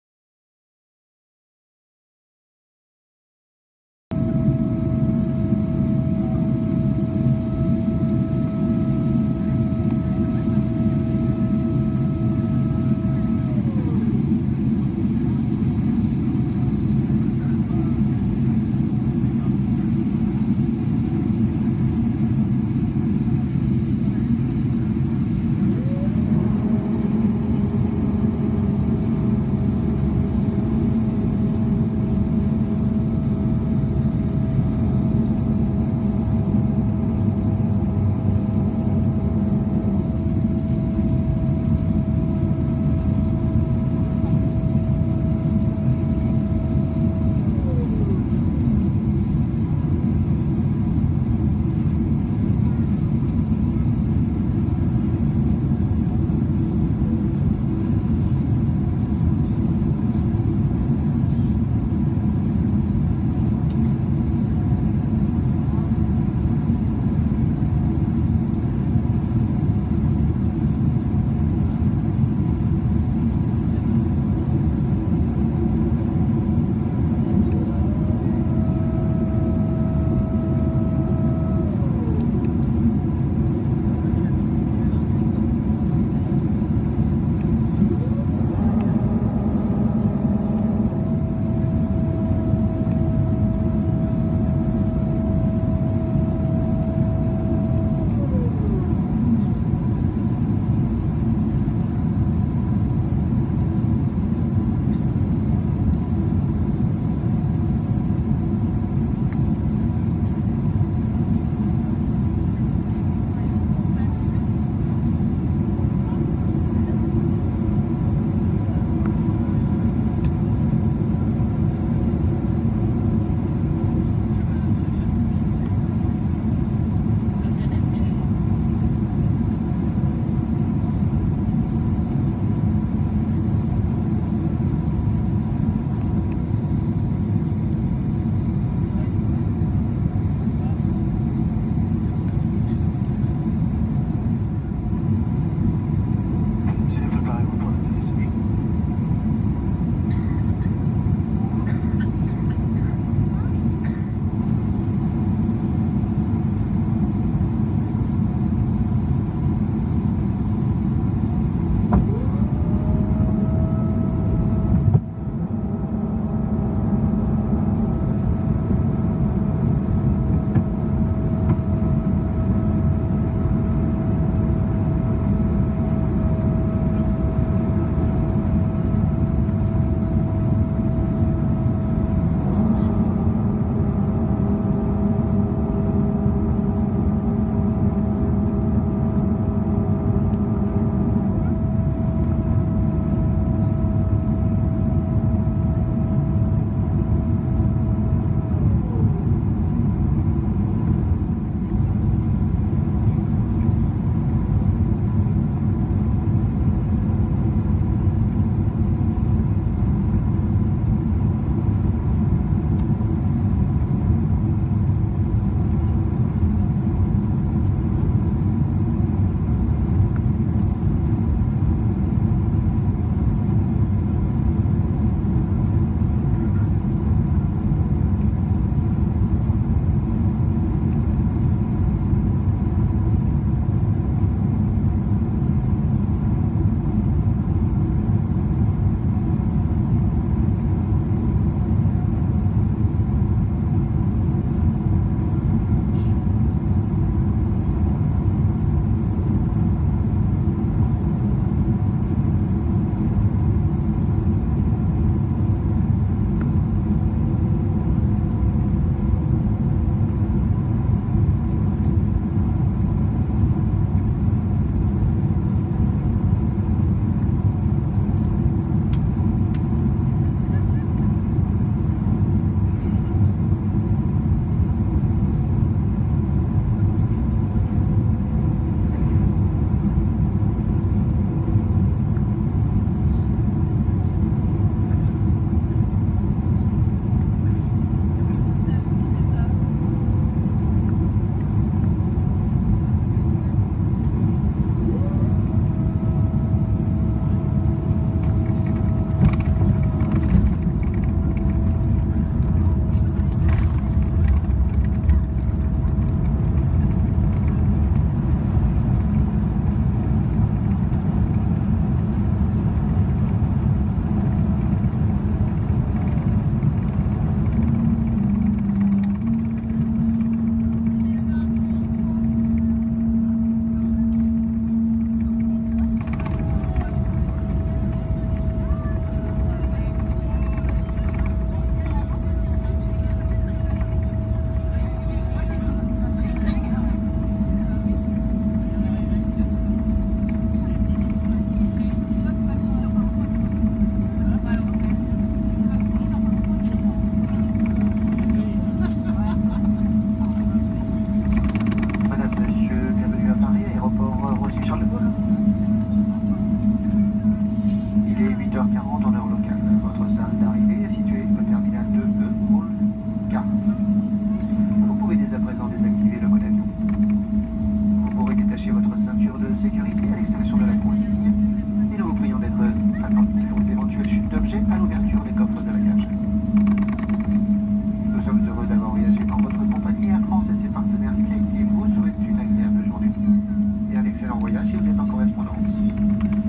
Air France Boeing 777 Landing in Paris CDG
Air France, Boeing 777-328 ER, F-GZNU, Landing in Paris CDG on runway 26L, Flight AF401 Santiago SCL - Paris CDG, Seat 32A, Saturday 18th February 2017.